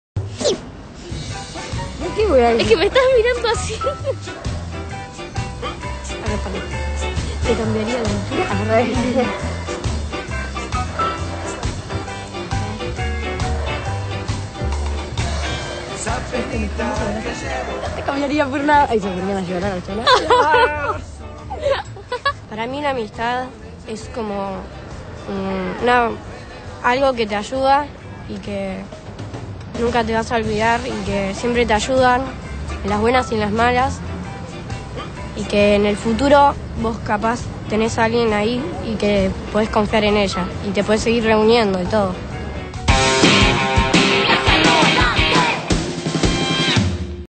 Caja rodante 28-06-10 - Amigas Georgina y Daiana
Dos amigas se encuentran para hablar del sentido de la amistad.